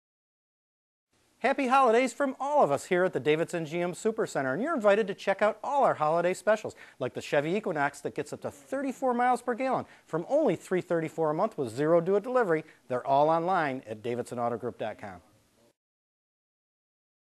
Davidson GM Super Center Holiday Deals on Equinox
New 2011 Chevy Equinox from only $334/month with $0 Due at Delivery.